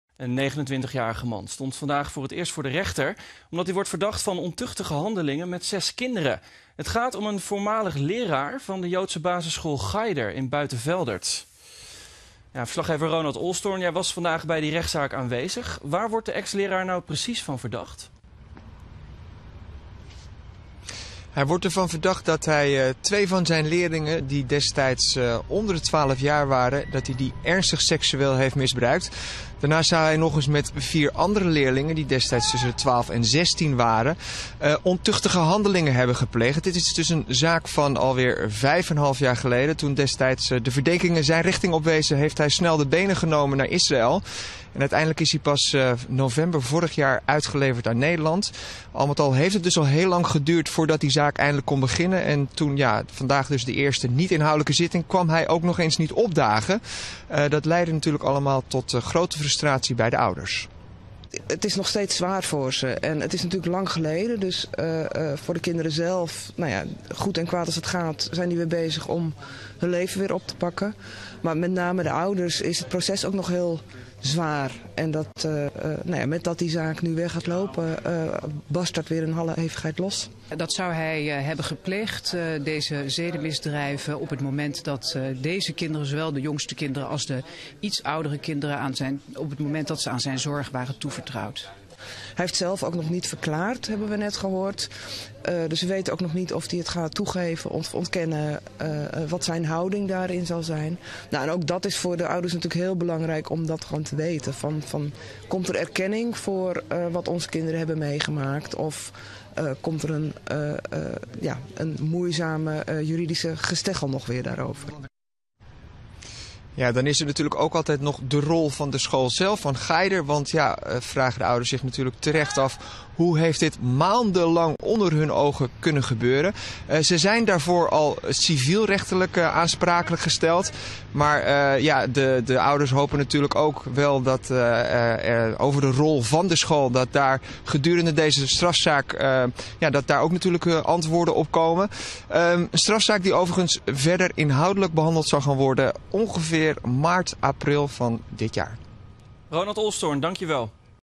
Van misbruik verdachte docent blijft zwijgen, tot frustratie van ouders
De oud-docent van de joodse school Cheider die verdacht wordt van seksueel misbruik en ontucht met zes kinderen blijft zwijgen tegen de politie en het Openbaar Ministerie. Dit tot grote frustratie van de ouders van de slachtoffers.

Ephraim S. (29) moest vandaag voor het eerst op een openbare zitting in de rechtbank verschijnen, maar kwam niet opdagen. Ook zijn advocaat was er niet.

De leraar wordt verdacht van seksueel misbruik van twee kinderen onder de 12 jaar en van ontucht met vier kinderen onder de 16 jaar.
Het misbruik zou zijn gebeurd in 2011 en 2012. Toen de verdenking bekend werd, vertrok hij naar Israël. Eind vorig jaar werd S. aan Nederland uitgeleverd. De verdachte zit in voorarrest, dat vandaag door de rechtbank met 90 dagen is verlengd.

Ontkennen of toegeven?
Volgens het Openbaar Ministerie ziet het er naar uit dat Ephraim S. wel een verklaring wil afleggen bij de rechter-commissaris. Het is nog onduidelijk of hij dan gaat toegeven of ontkennen.

Dat is voor de ouders heel belangrijk om te weten, zegt Margreet de Boer, advocaat van vijf van de zes slachtoffers. 'Wordt het een moeizaam proces of gaat hij feiten bekennen?'

'Kinderen pakken leven weer op'
Het vermeende misbruik vond jaren geleden plaats. Ook duurde het lang voor de leraar kon worden uitgeleverd. Volgens De Boer pakken de kinderen hun leven weer op, 'zo goed en zo kwaad als dat gaat'. Voor hun ouders blijft de situatie nog heel zwaar. Ze vragen zich ook af of de school wel zorgvuldig genoeg is geweest. De Boer: 'Er zat met name veel tijd tussen het melden en de aangifte.'

Het onderzoek van de politie en het Openbaar Ministerie is klaar. Toch zal de inhoudelijke behandeling van de zaak waarschijnlijk pas na de zomer beginnen. Dat komt doordat de verdediging nog onderzoekswensen heeft. De advocaat van de verdachte wil een groot aantal getuigen verhoren en een aantal deskundigen.